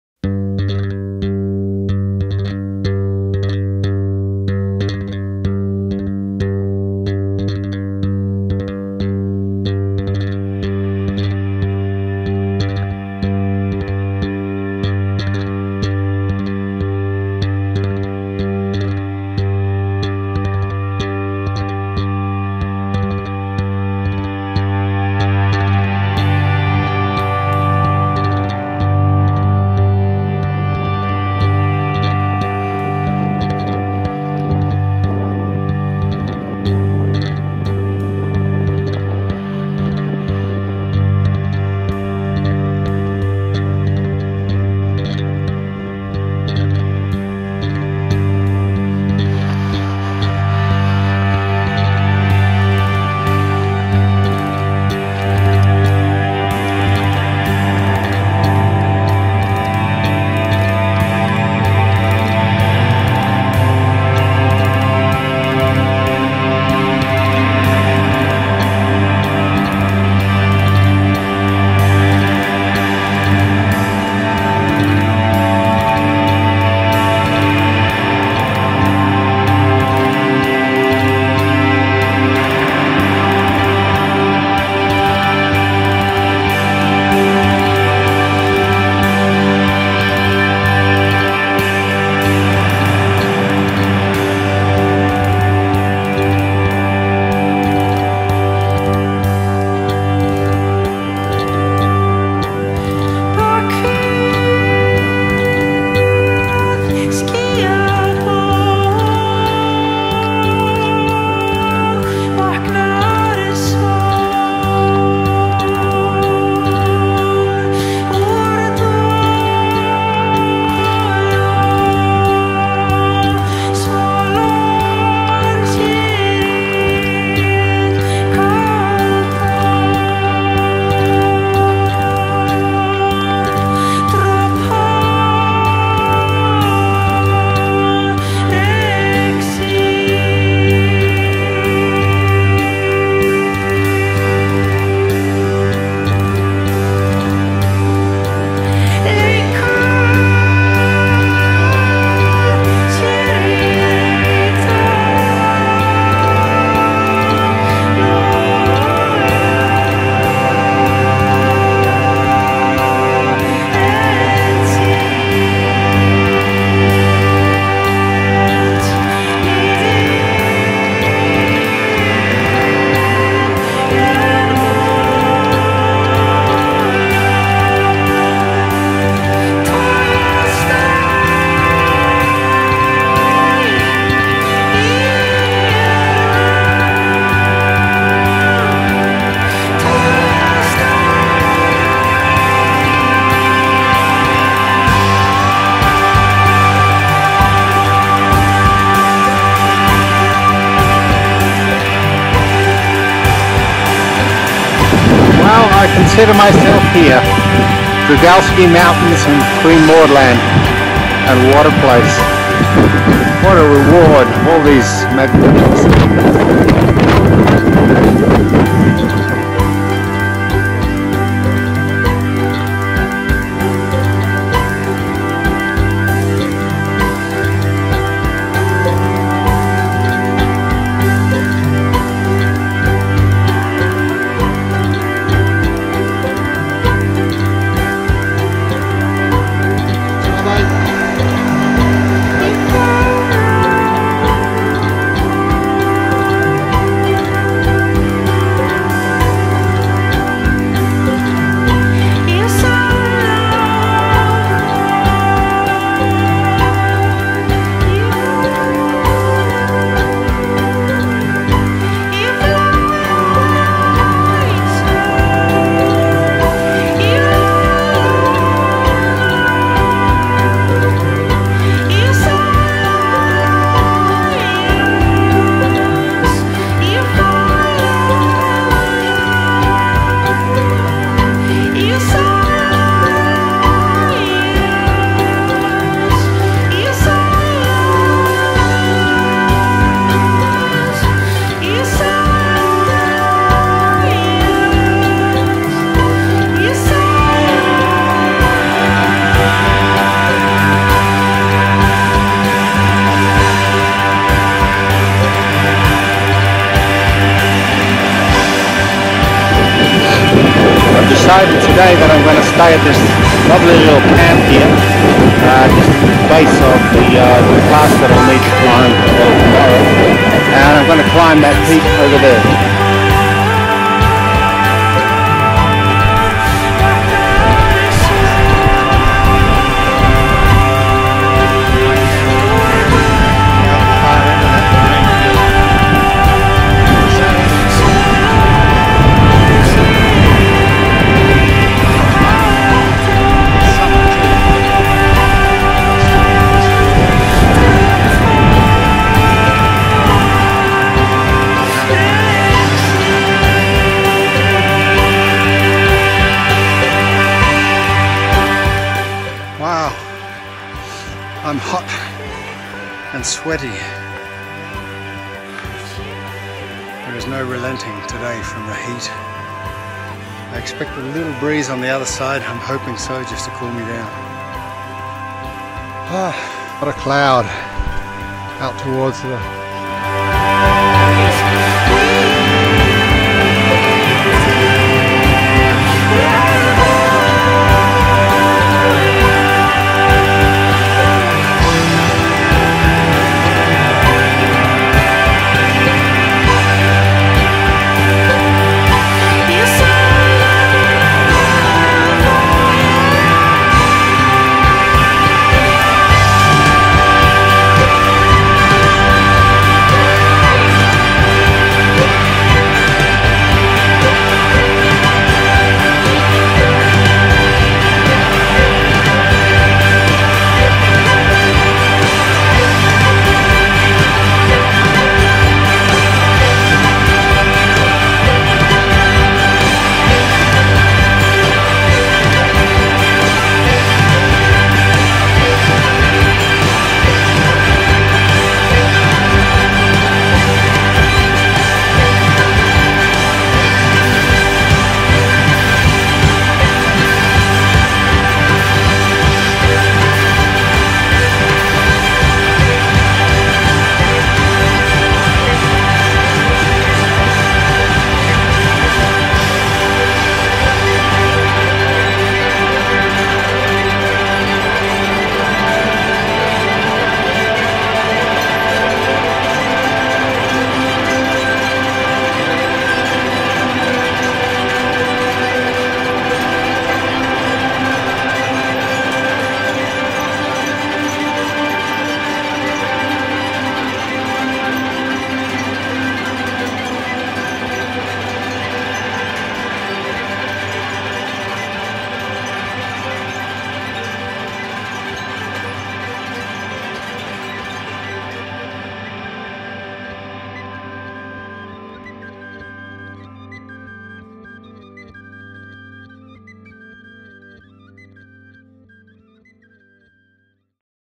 Kite-Ski Ulvetanna. Antarctica
In December 2015 polar adventurer and guide Eric Philips kite-skied solo to Ulvetanna, an iconic mountain in the Drygalski Mountains of Queen Maud Land, Antarctica